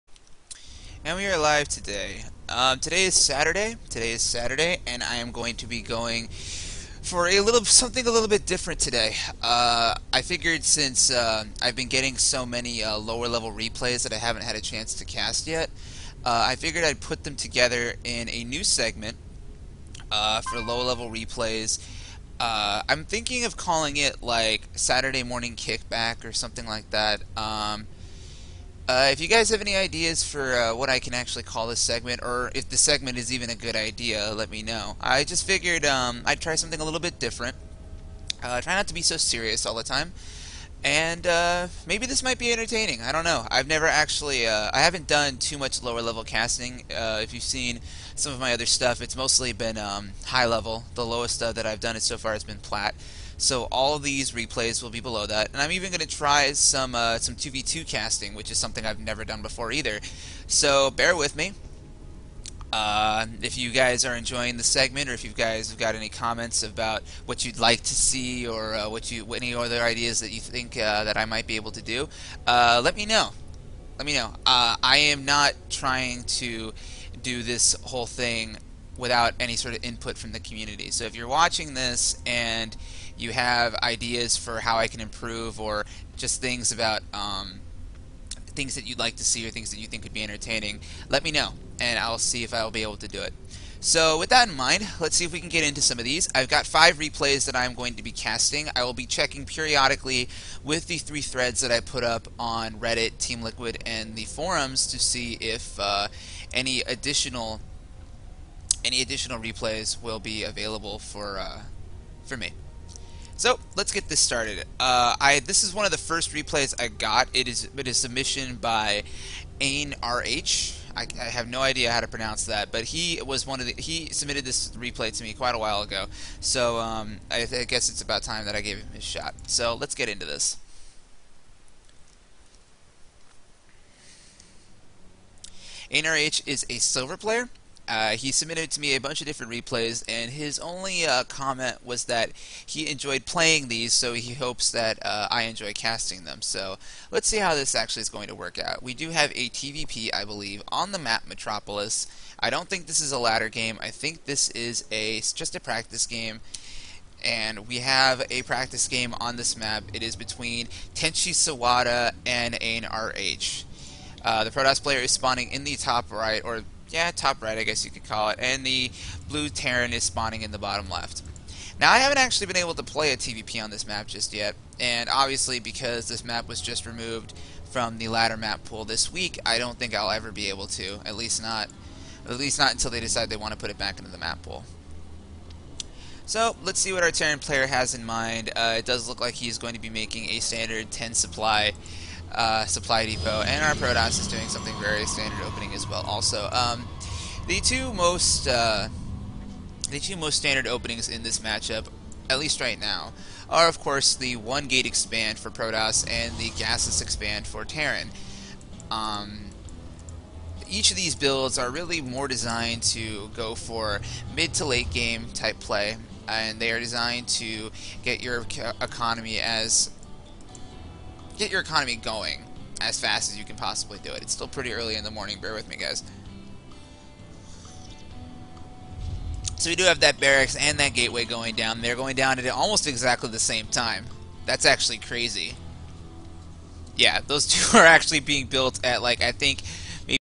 Saturday Morning Kickback #1 1/2 Archon rush
Introduction to my new Saturday Morning Kickback segment -- Recorded via twitch.tv. If you'd like to see more of my live casts please follow me: @ktvindicare on twitter to find out when I go live.